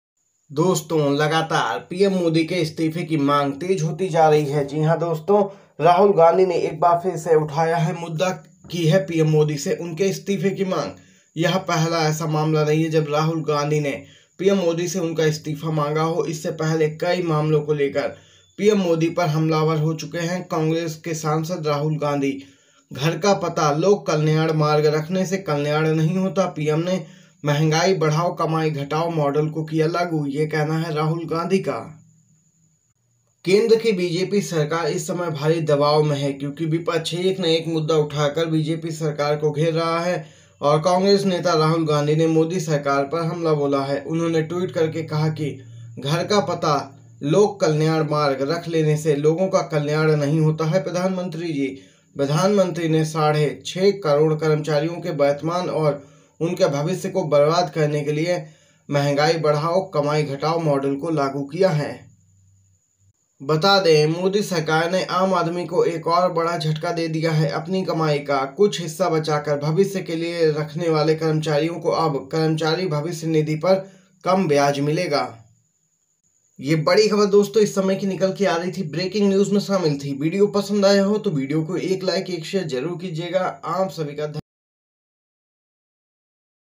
राहुल गांधी की बड़ी जीत इस्तीफा देने को तैयार मोदी ? सीडी हो गई लीक बड़ी खबर
नमस्कार दोस्तो आपका स्वागत है HRI NEWS पर 🙏🙏 बड़ी खबर इस समय की देखे ,धन्यवाद
HRI NEWS एचआरआई न्यूज़